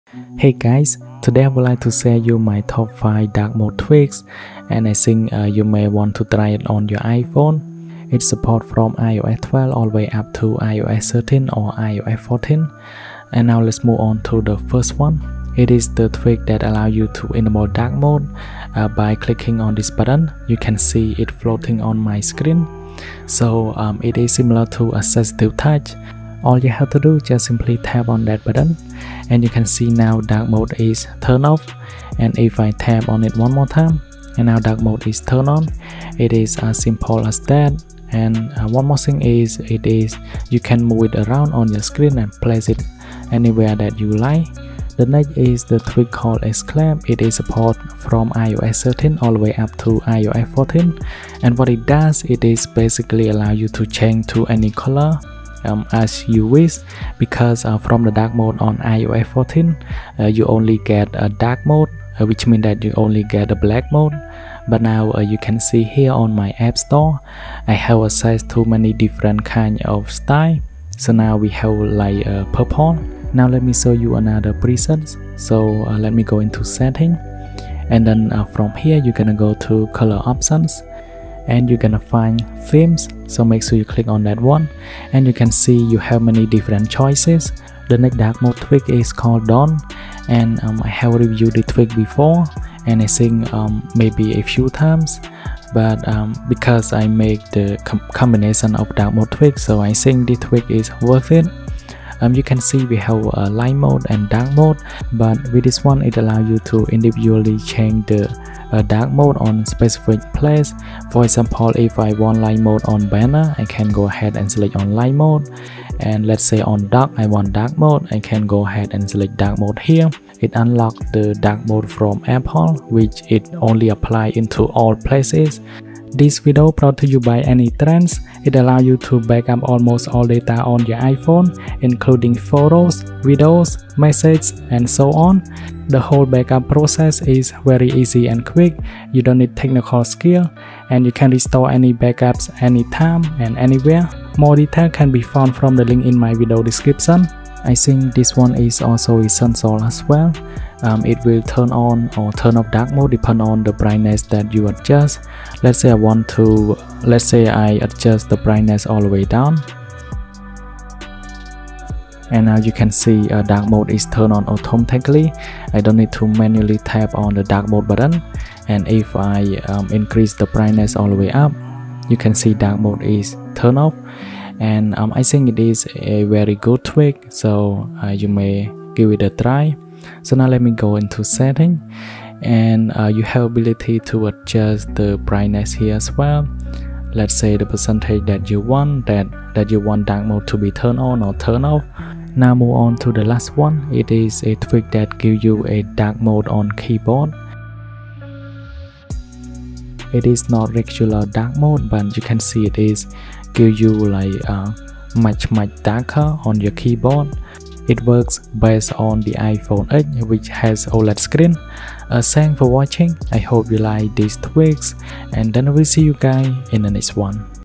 Top 5 Amazing Dark Mode Tweaks for iOS 12/13/14
Timestamp:
00:13 FloatingDarkMode
00:45 Eclipse Dark Mode iOS13+
01:28 Dawn
02:29 DarkSwitch
03:13 Dark Keys Reborn